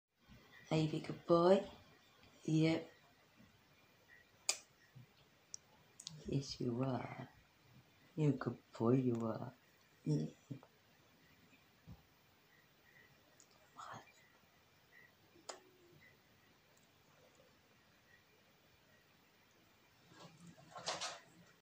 our golden tegu enjoying some cuddles
My big male Golden tegu enjoying some loves and affection 🥰
It took us a while but we got there in the end ❤️